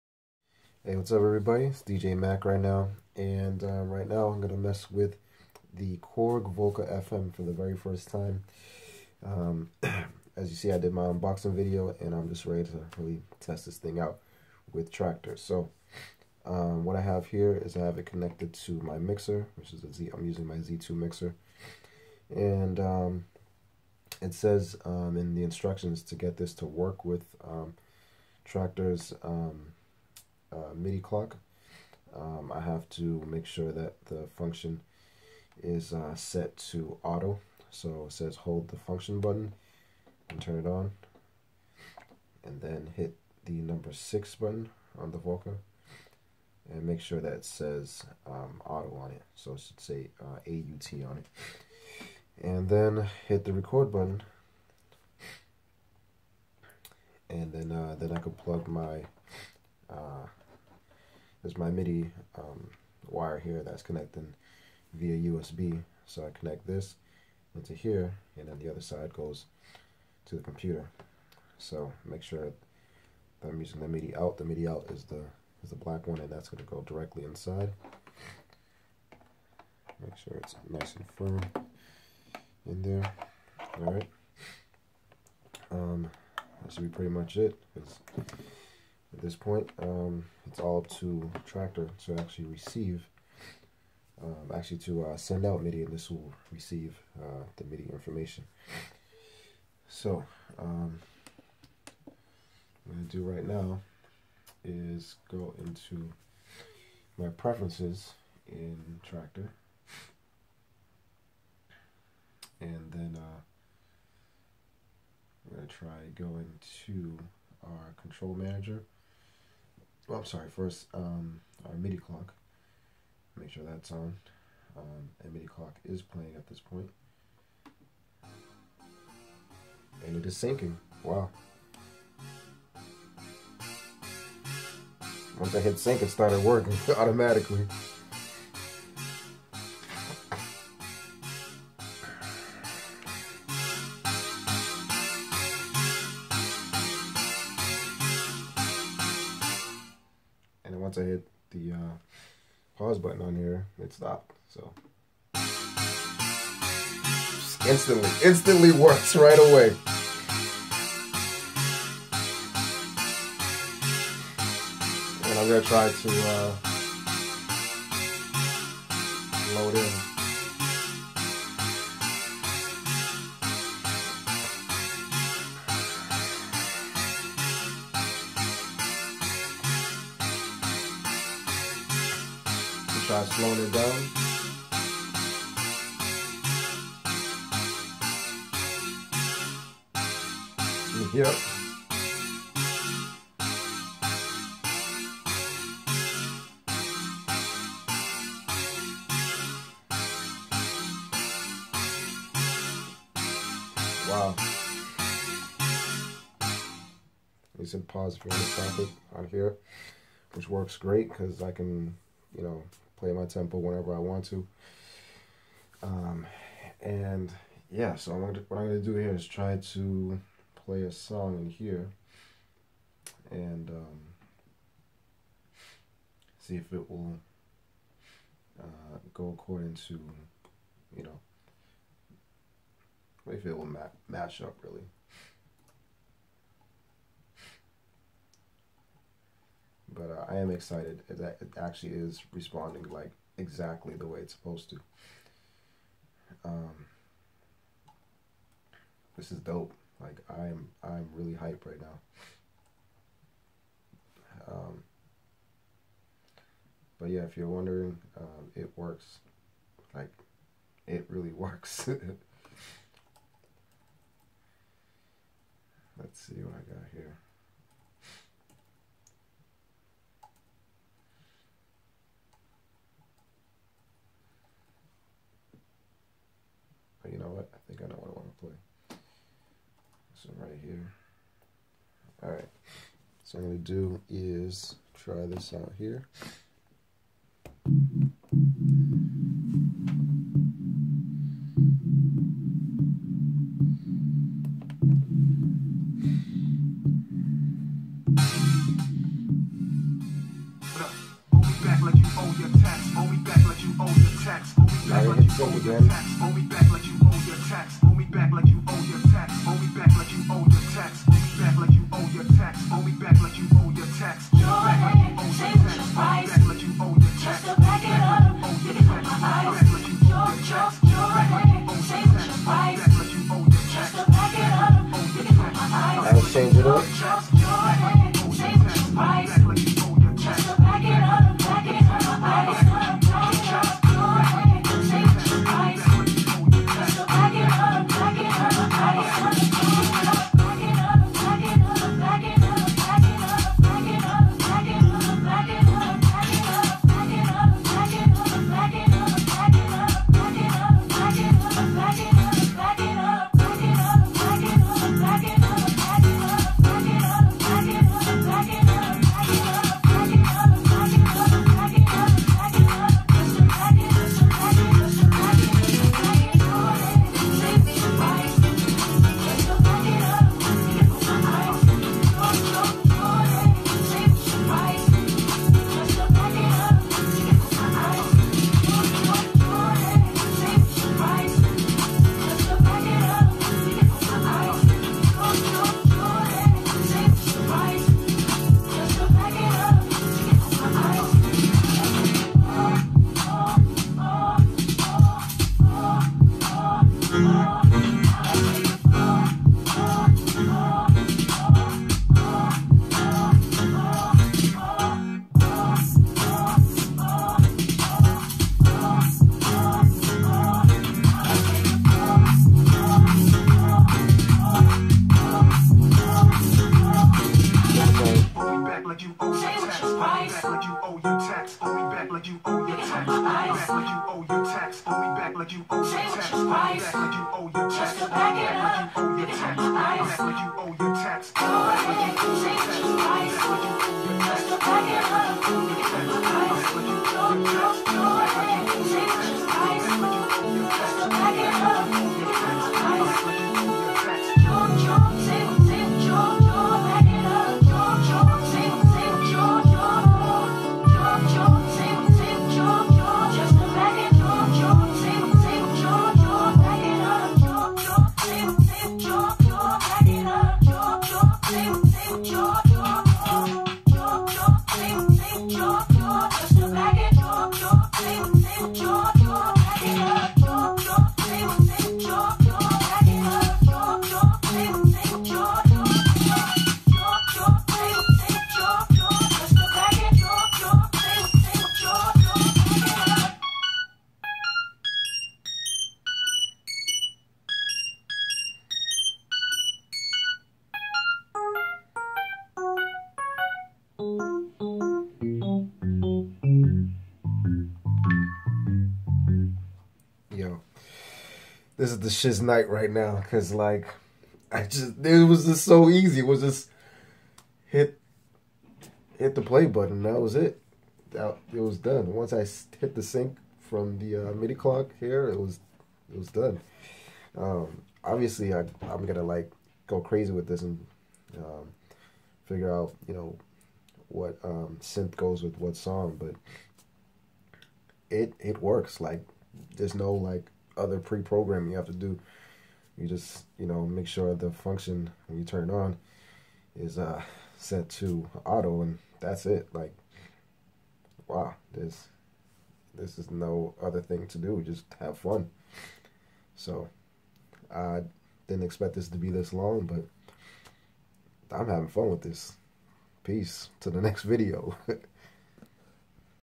Korg Volca FM sync with traktor scratch DJ Mac
How to sync Volca with Traktor.

Note:Be sure to plug the volca USB to MIDI directly to the computer, not a hub!!!

1. Turn off volca
2. Hold function button on volca while holding the volca power on button. Then release when on.
3. 6 key once or twice to see AUT.
4. Press rec. Button.
5. In traktor got to preferences
6. Then go to MIDI clock and turn on MIDI Clock
7. In up right corner in traktor press the metranome(in full view)
8. Click auto and the play button under sync and booom! Your off!!
9. Play songs and hit sync if songs are off with the volca.
HAVE FUN😉😉😉😉😀😀